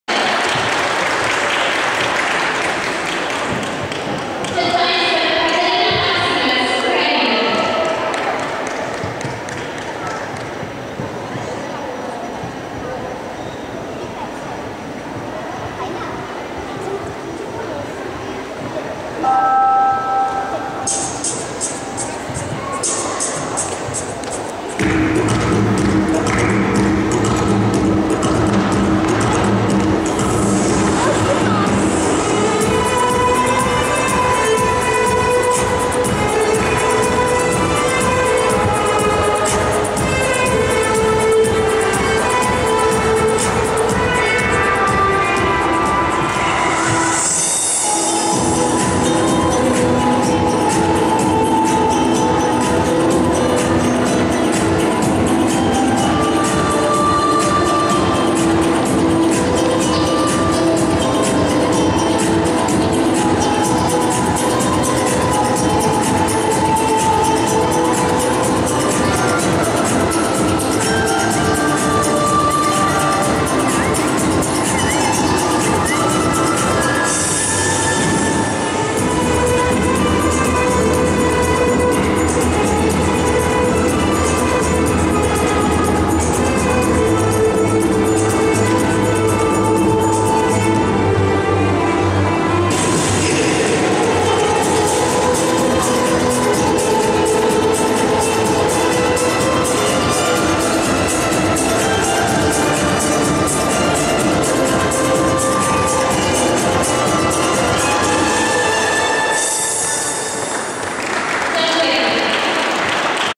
Hoop Wroclaw, Poland 20-21 June 2014
Kateryna Maksymets, 2005, XXXIII Tournament in Rhythmic Gymnastics, Wroclaw, Poland 20-21 June 2014, hoop